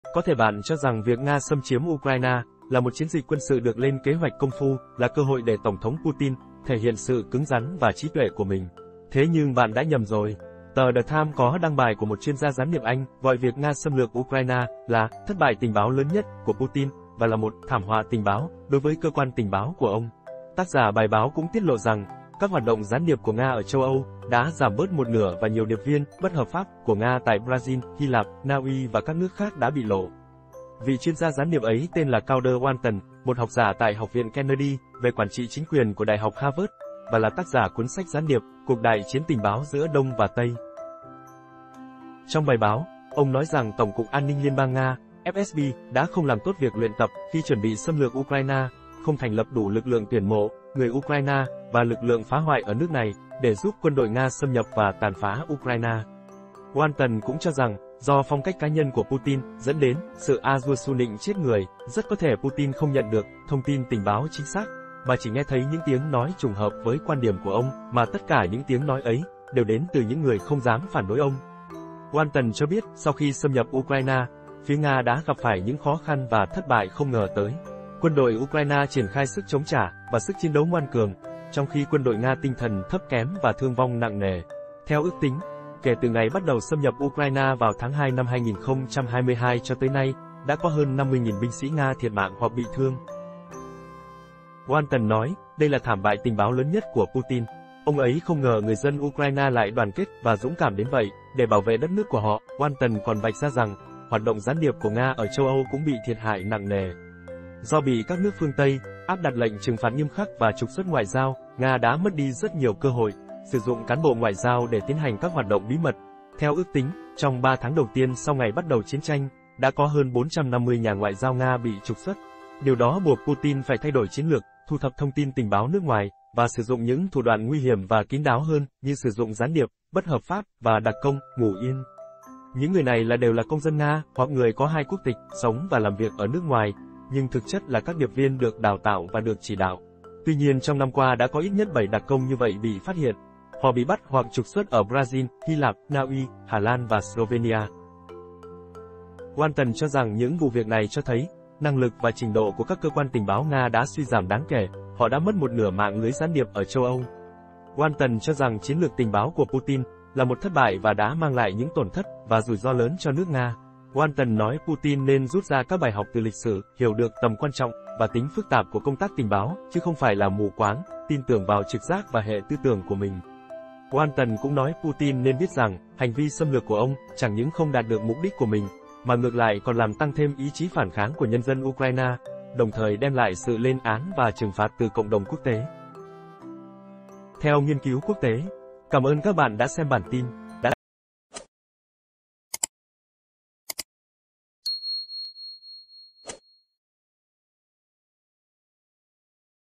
CÔNG TÁC TÌNH BÁO - THẤT BẠI LỚN CỦA PUTIN
Có thể bạn cho rằng việc Nga xâm chiếm Ukraine, là một chiến dịch quân sự được lên kế hoạch công phu, là cơ hội để Tổng thống Putin, thể hiện sự cứng rắn và trí tuệ của mình. Thế nhưng bạn đã nhầm rồi. Tờ The Times có đăng bài của một chuyên gia gián điệp Anh, gọi việc Nga xâm lược Ukraine, là "thất bại tình báo lớn nhất" của Putin, và là một "thảm họa tình báo", đối với cơ quan tình báo của ông. Tác giả bài báo cũng tiết lộ rằng, các hoạt động gián điệp của Nga ở châu Âu, đã giảm bớt một nửa và nhiều điệp viên, "bất hợp pháp"của Nga tại Brazil, Hy Lạp, Na Uy và các nước khác đã bị lộ.